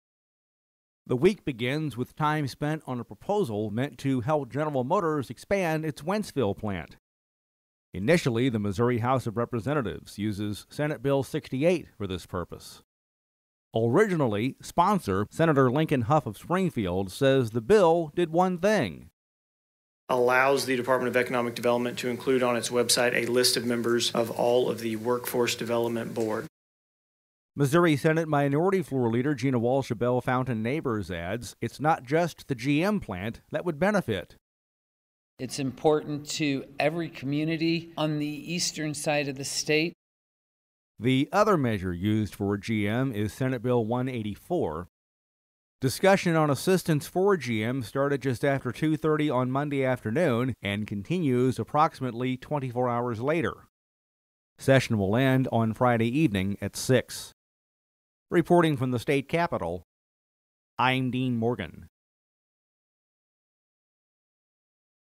The Missouri Senate Minute for May 14: GM Proposal
JEFFERSON CITY — Discussion continues on the floor of the Missouri Senate over how to help General Motors potentially expand its plant in Wentzville. The answers may be found in either Senate Bill 68 or Senate Bill 184.